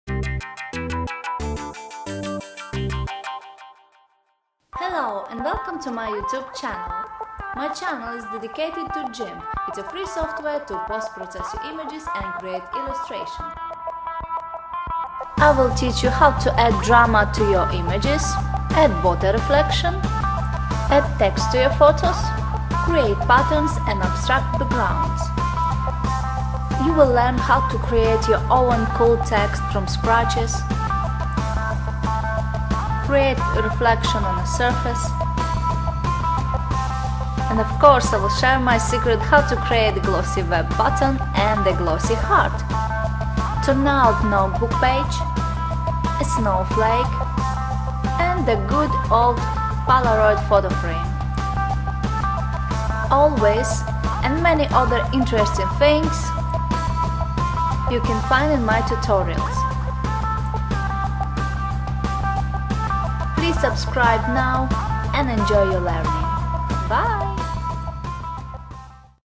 Welcome to my Youtube channel
GIMP is a free software and it can be downloaded

This channel is for everyone who is interested in learning how to post process images and create illustrations in GIMP.  This software gives freedom to your creativity and imagination.  My tutorials are for GIMP users with different level of skills, including the beginners.